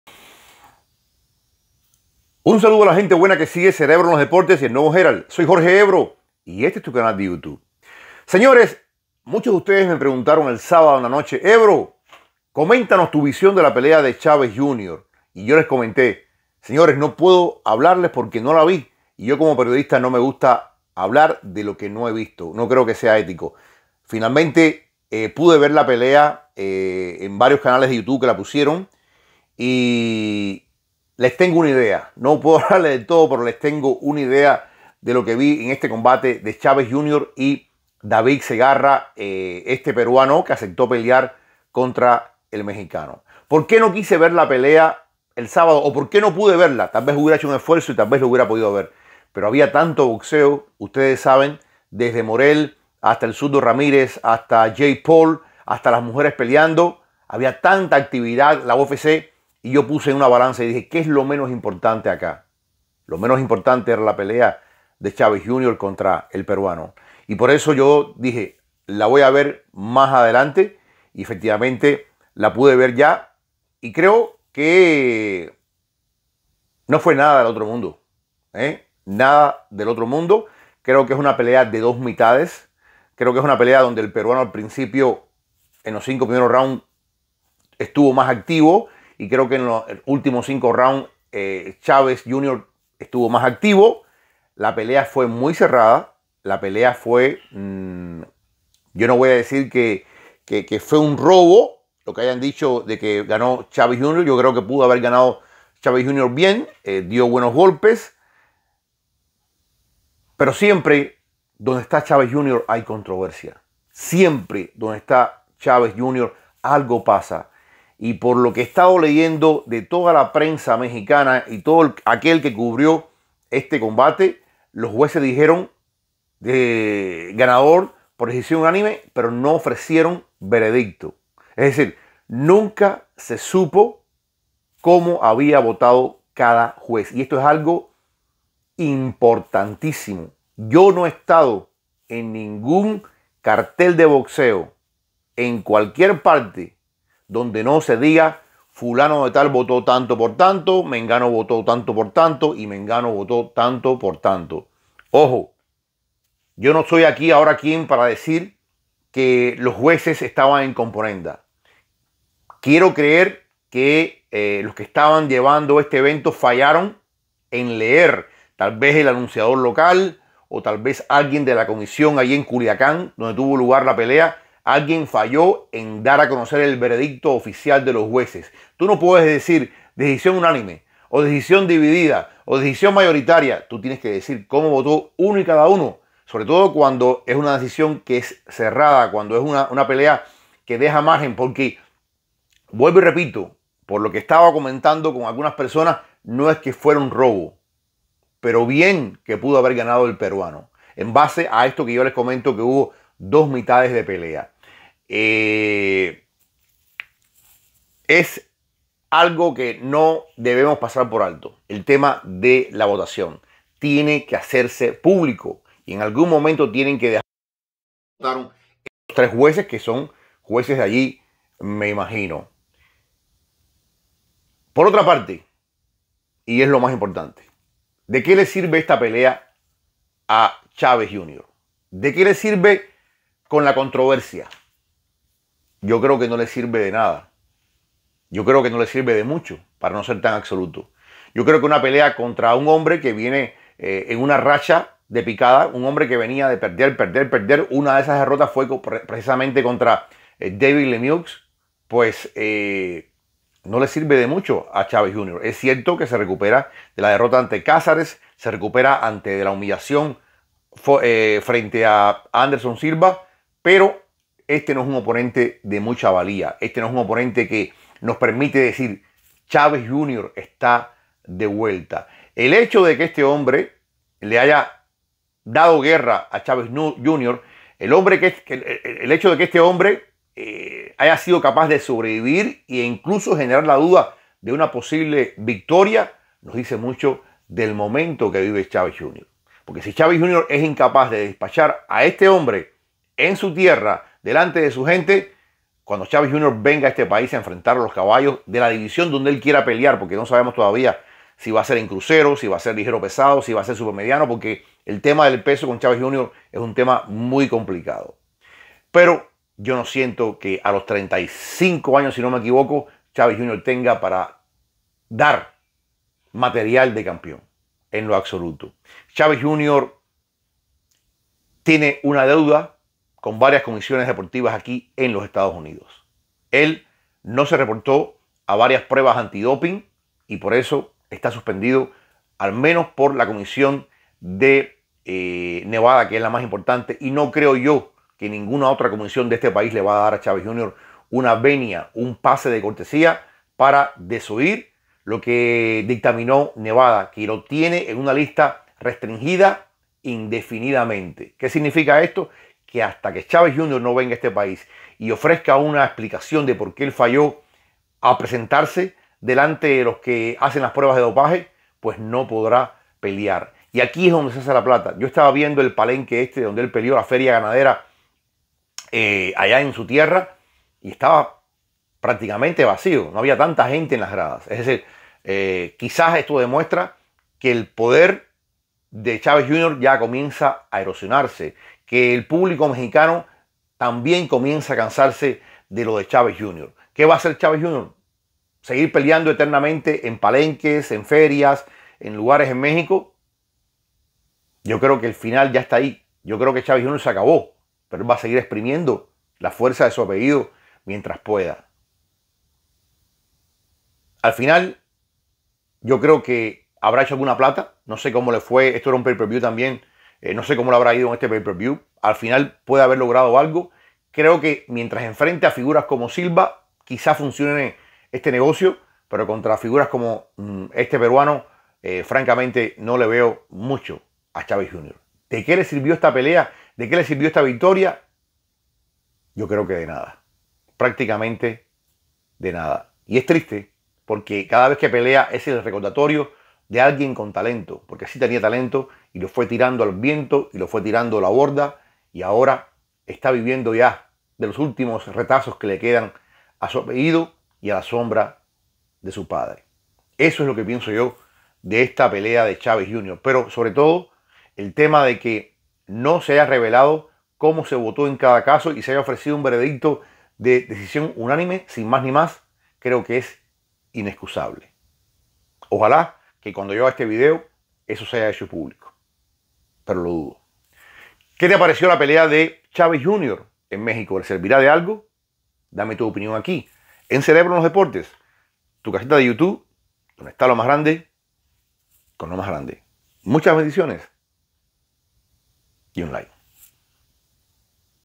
JULIO CESAR CHAVEZ JR, el drama no le abandona
Un regreso al ring que planteó dudas en la decisión final fue la que tuvo el boxeador Julio César Chávez Jr., quien derrotó al peruano David zegarra, en una pelea en Sinaloa.
Julio César Chávez Jr. triunfó sobre David Zegarra. Durante el certamen que se llevó a cabo en el Palenque de la Feria Ganadera de Culiacán, el mexicano superó a su oponente por decisión unánime, pero la polémica estuvo presente porque las tarjetas de los jueces no fueron dadas a conocer.
La pelea se pactó en 10 episodios y en los primeros cinco el peruano logró contener a Julio César Chávez Jr., quien no pudo encontrar la fórmula para dominar a su rival sudamericano.
Para los siguientes episodios, el mexicano logró igualar el proceso, pero no tuvo un control claro sobre David Zegarra y al final se determinó que Chávez Jr. ganó por decisión unánime.